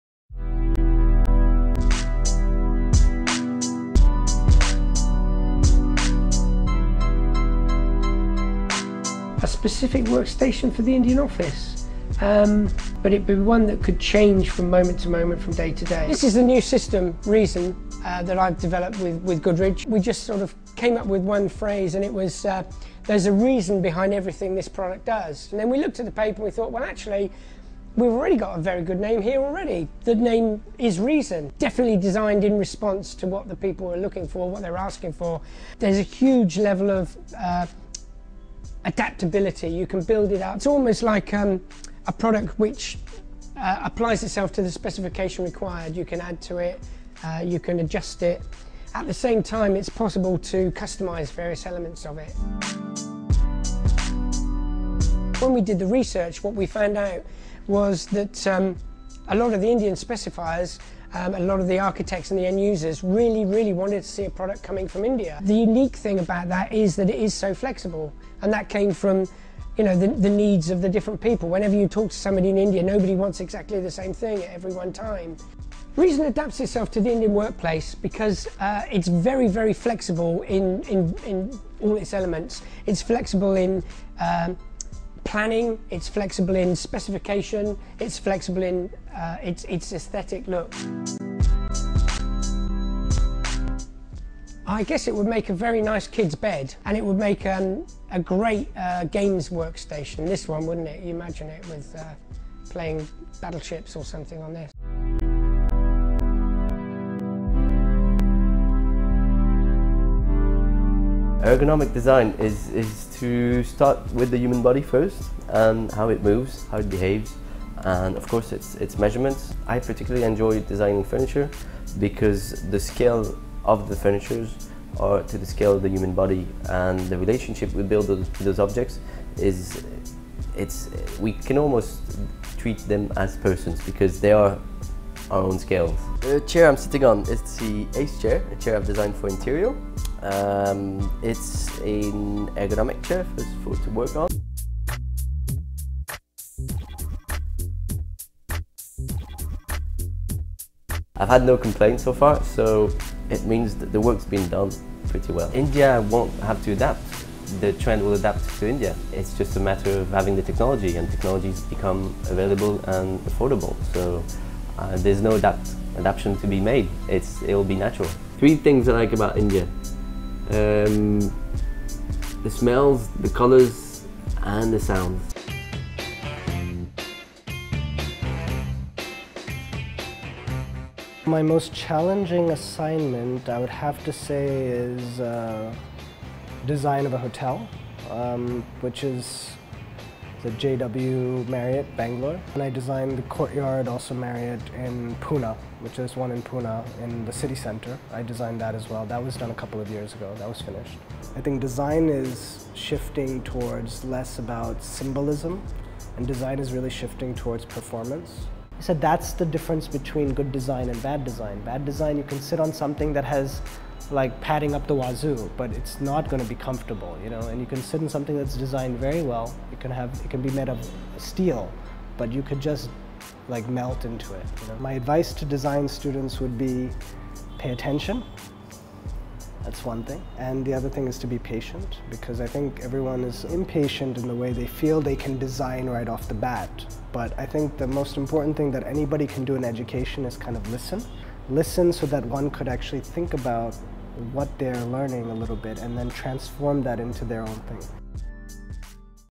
Godrej Interio Designer Diaries
There is a growing need for international-class office furniture and allied workspaces that can cope with the pace of growth unique to the Indian market. Here’s an interview with the well-known designers Antoine Phelouzat and Roger Carr talking about their design for Interio’s office furniture.